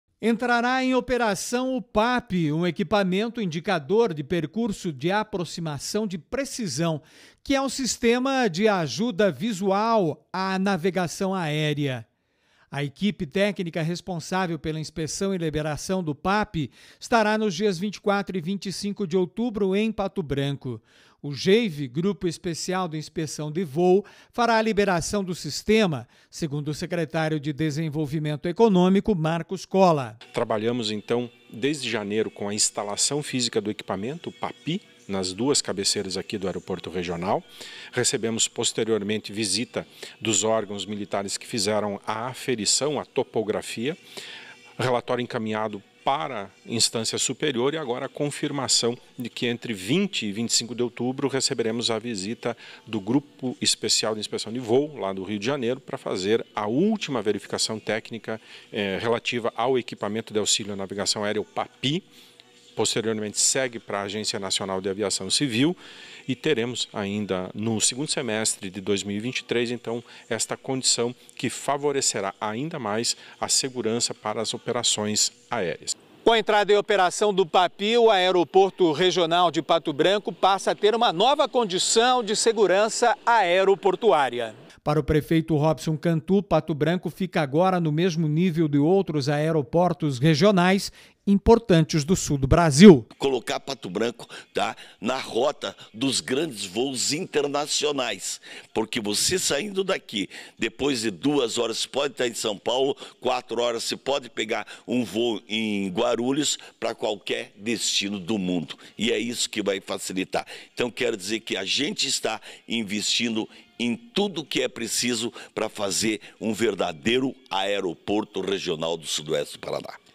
PAPI entra em operação no Aeroporto Regional de Pato Branco
Atualidade: O PAPI, um equipamento indicador de percurso de aproximação de precisão, sistema que auxilia a navegação aérea, entrará em operação.